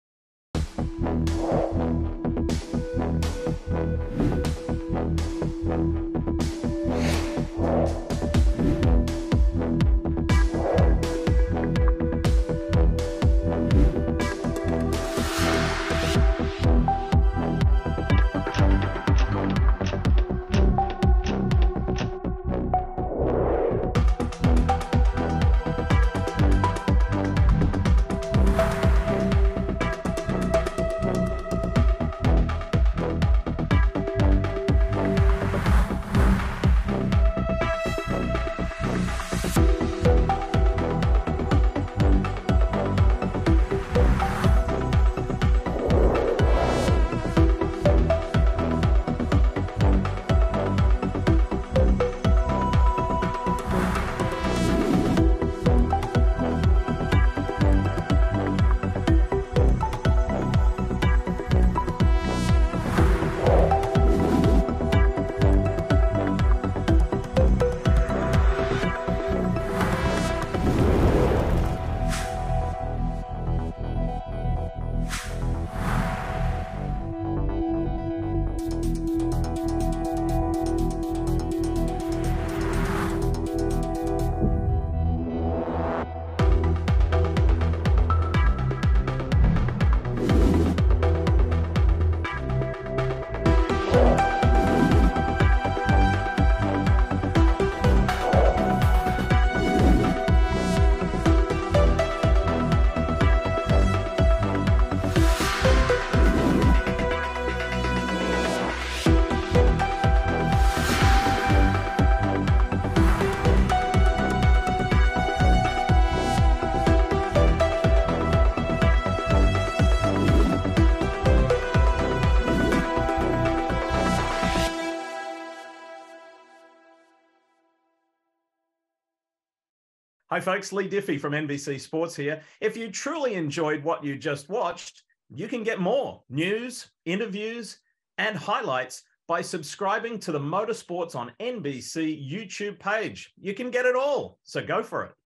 Ever wonder the difference between a MotoGP bike and a regular motorcycle? | Motorsports on NBC
Check out all of the characteristics that make a MotoGP bike so unique compared to other motorcycles.